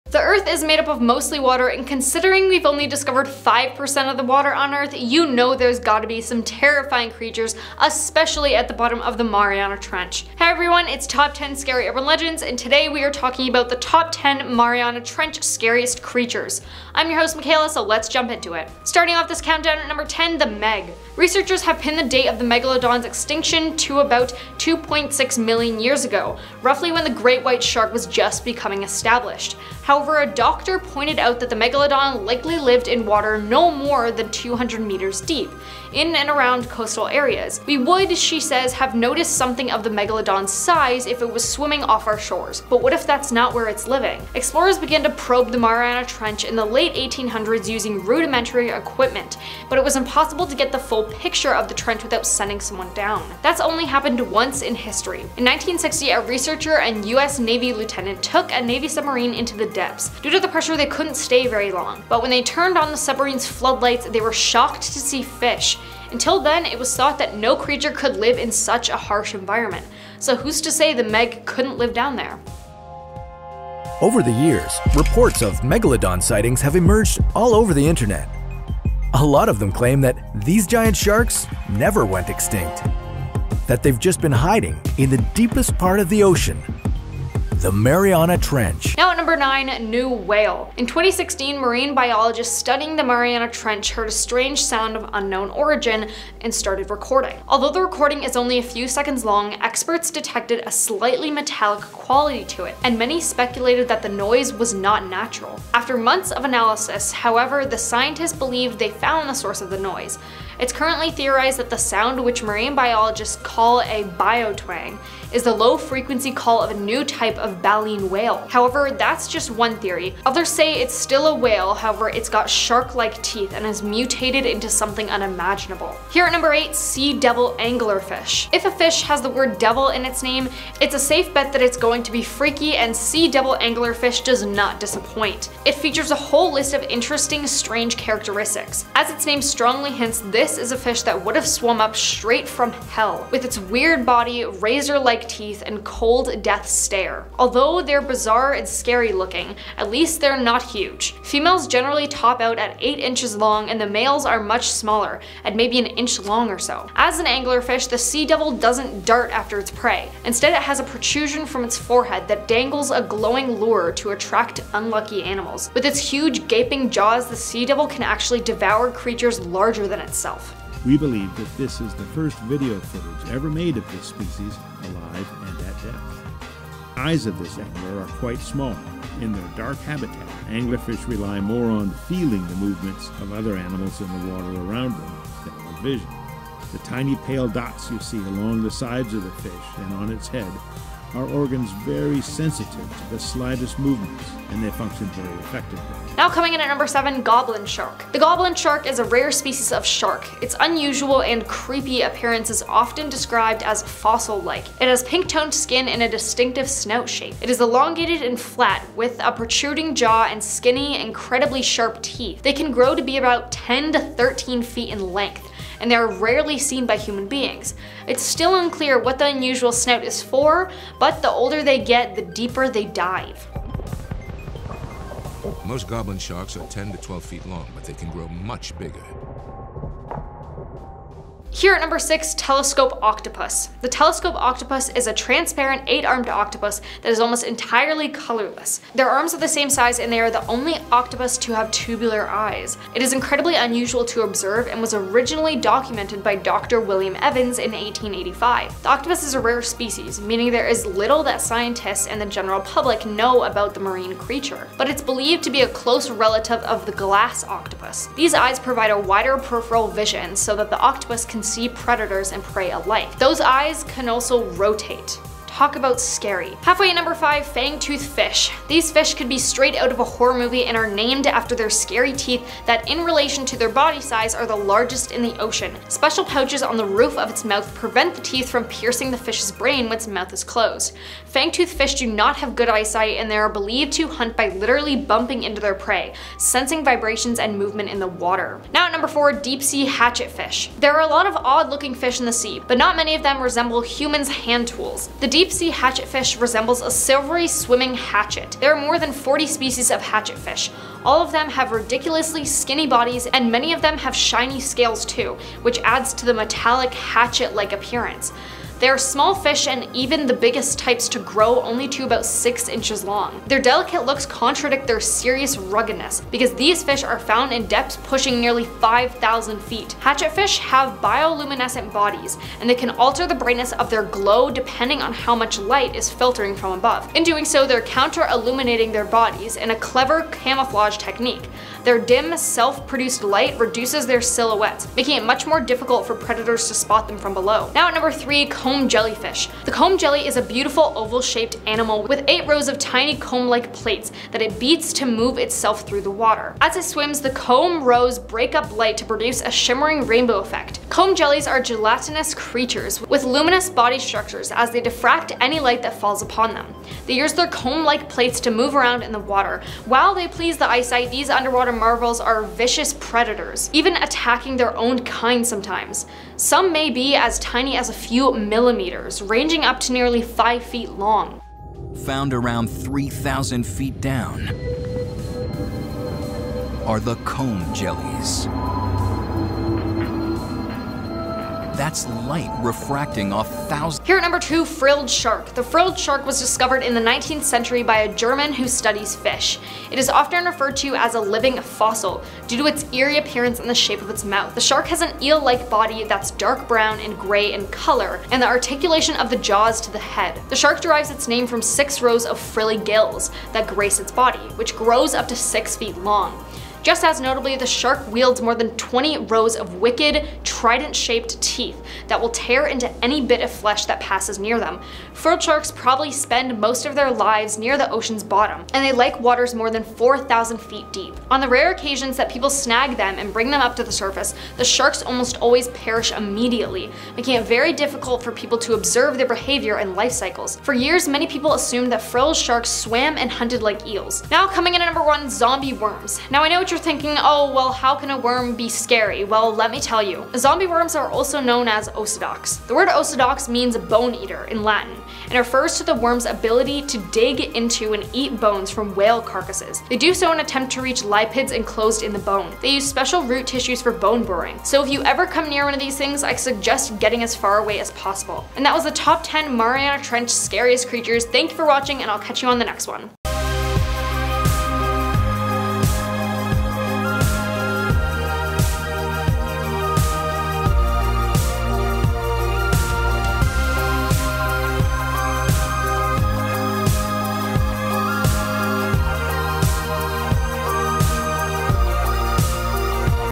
Top 10 Mariana Trench Scariest Creatures
In this video you will encounter the most creepy, scary ocean stories from reddit. This scary video will  include some of the scariest sea stories from the Mariana Trench with horrific creatures. You will want to ask reddit what else lives in the deep ocean, but I warn you to watch with caution because this video is not for everyone!

Host By: Makayla McLaughhlin
Edited By: Simon Clifford: @simonmclifford